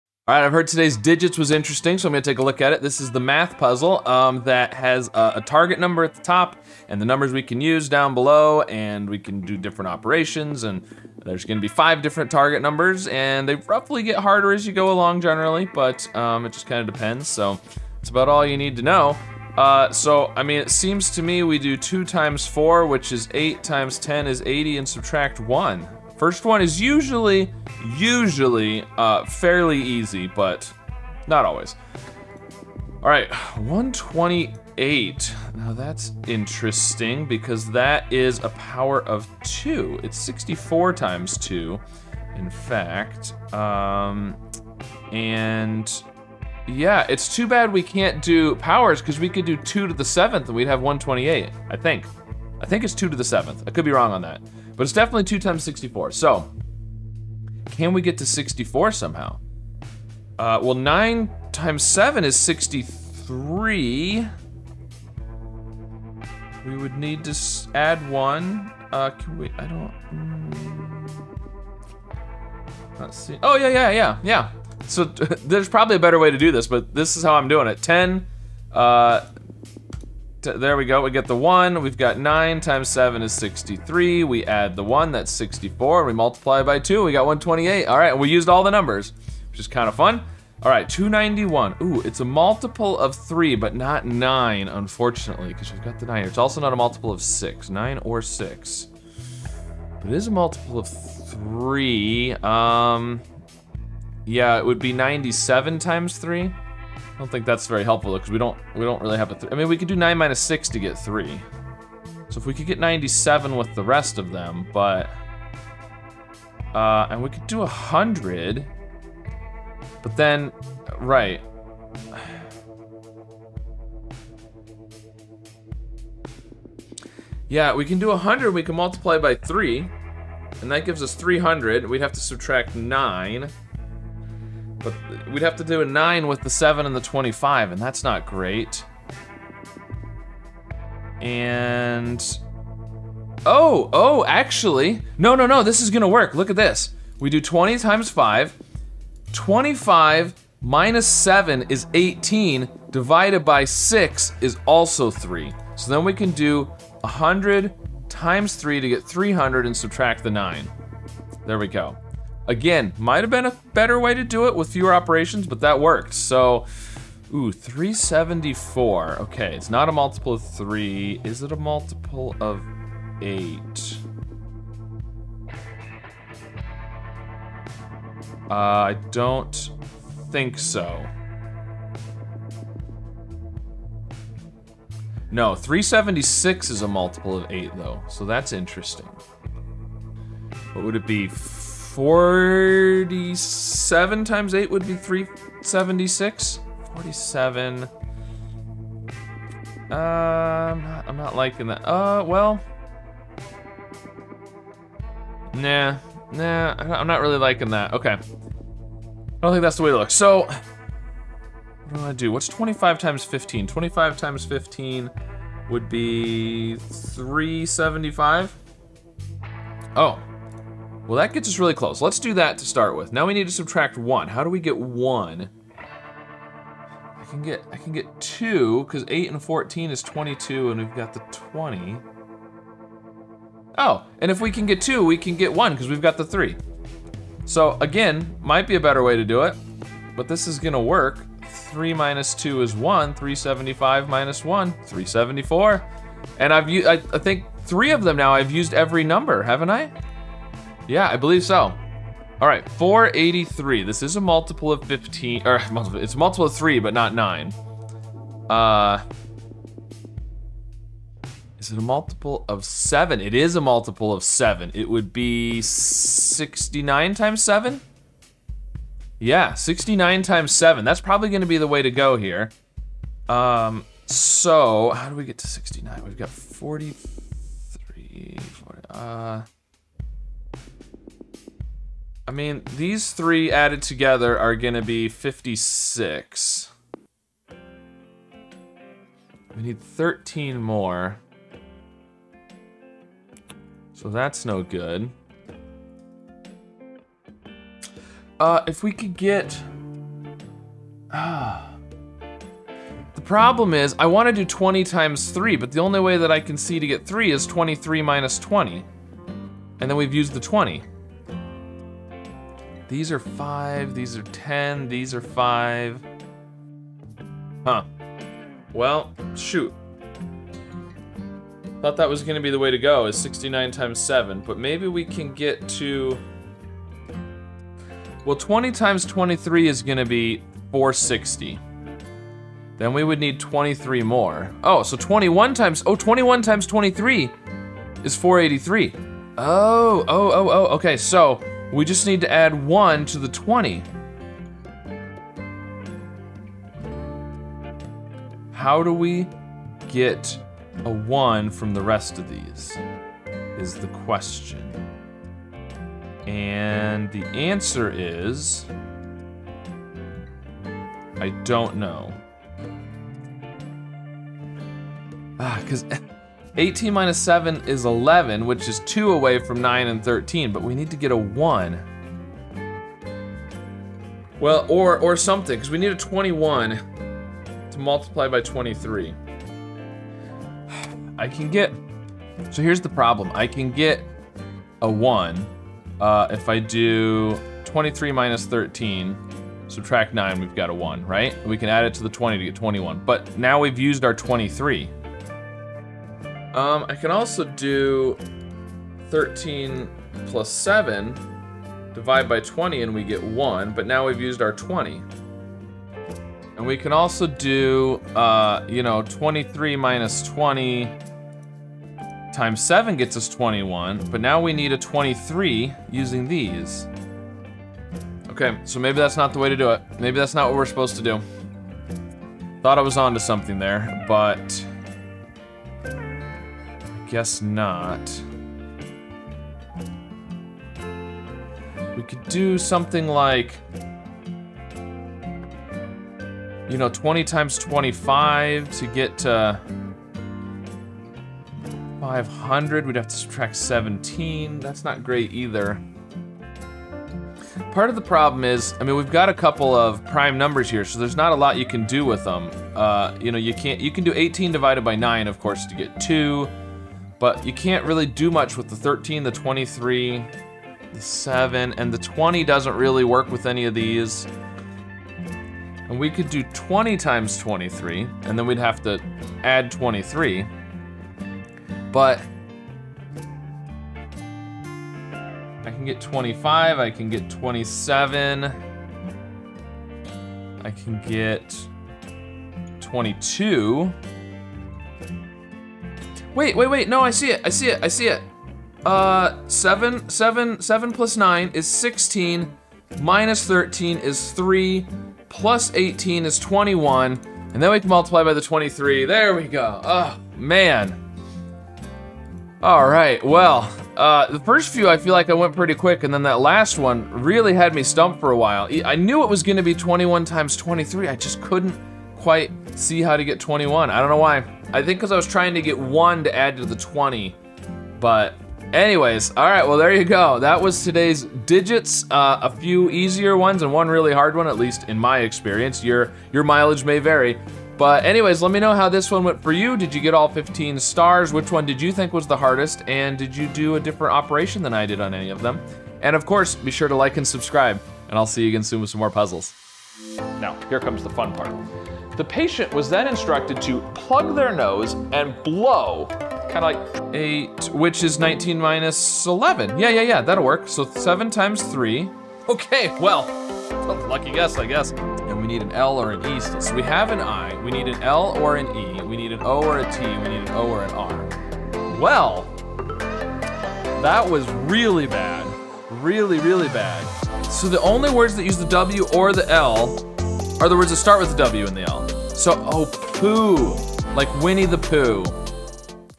There's always one...
I'm Scott Strosahl, and today I'm solving the 'Digits' puzzle for June 15th.